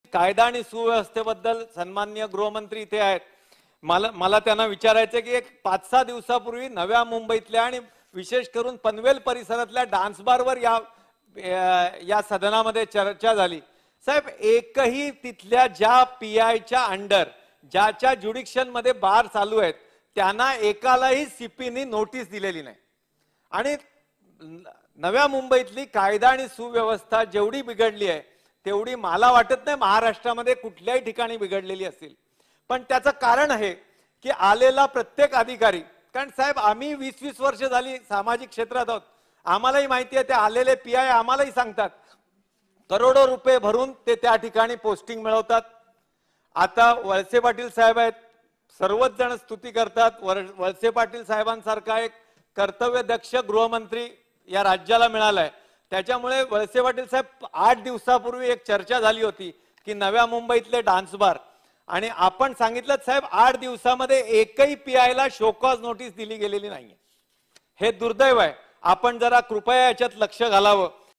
नवी मुंबईत कायदा आणि सुव्यवस्थेचे वाजले तीन-तेरा
नवी मुंबईत डान्स बार संदर्भात आठ दिवसात सिपिने एकही नोटीस दिली नाही हे दुर्दैवी